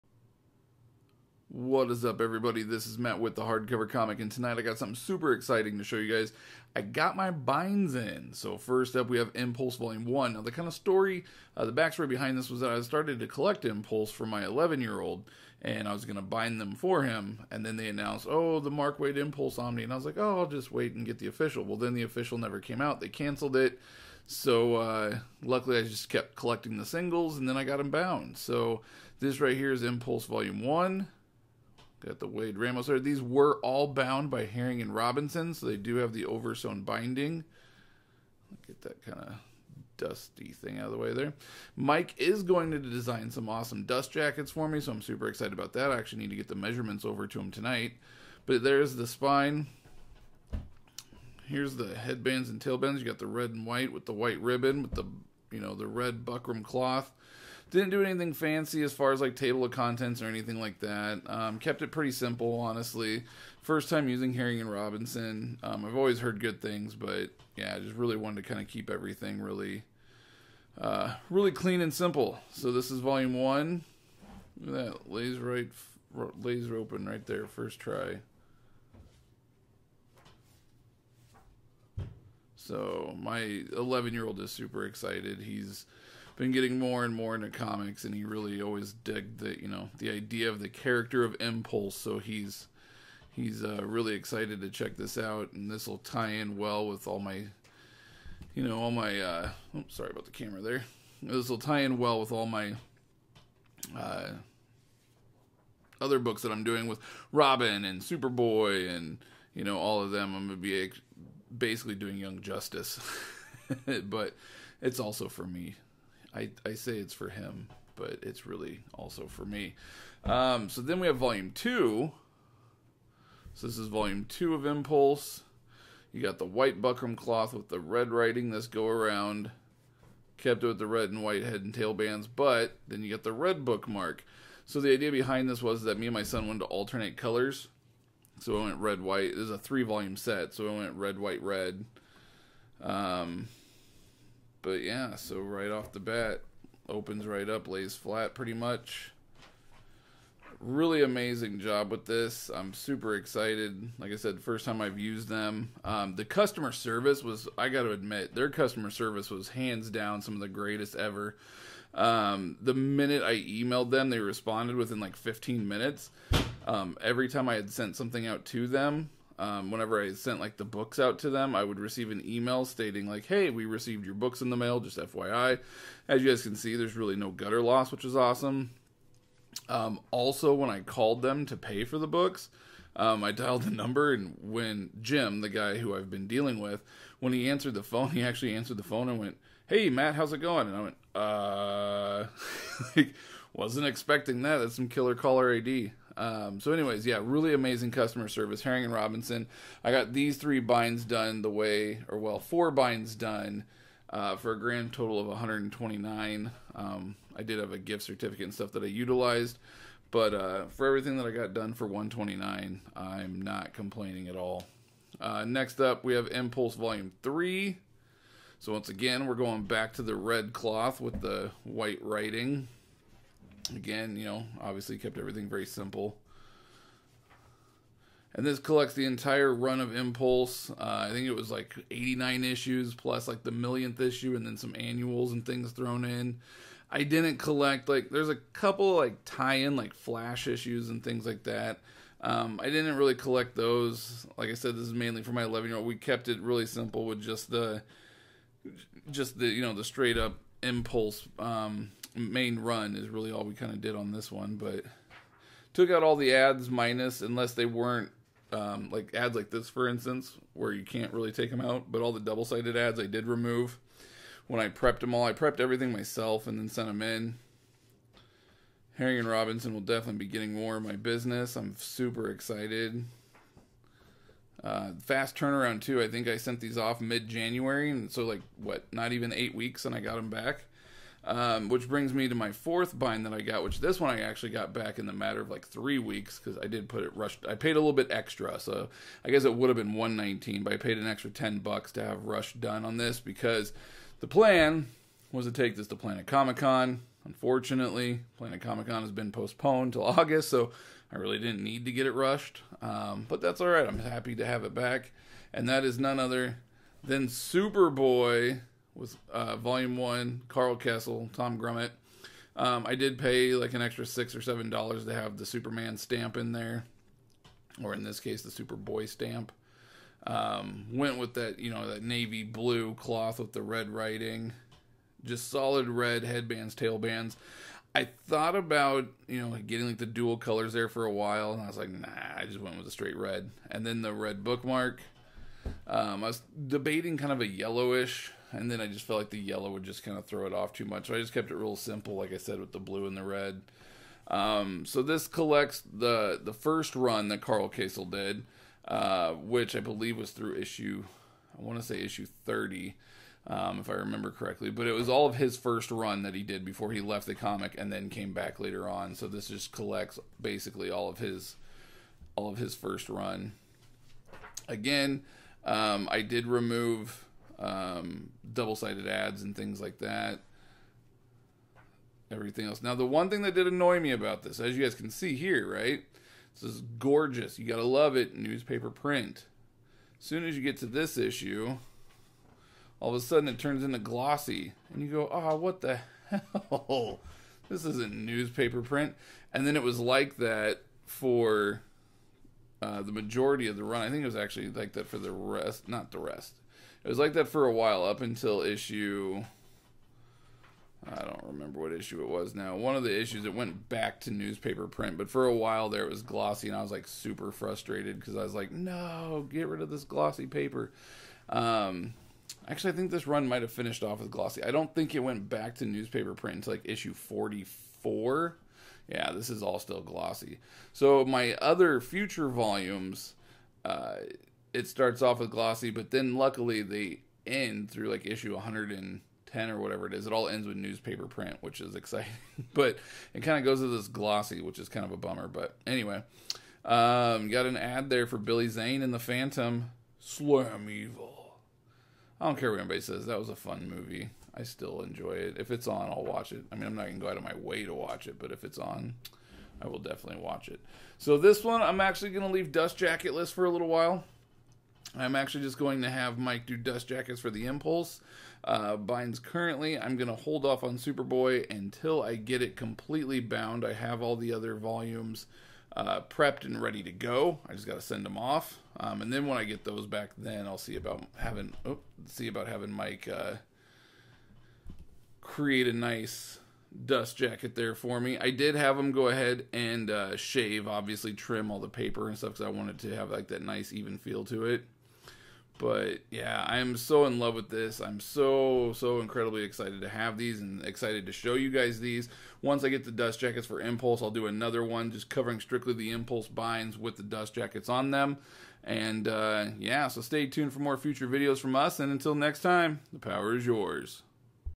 NEW Custom Binds! Impulse, Superboy
Matt shows you his latest custom bind haul!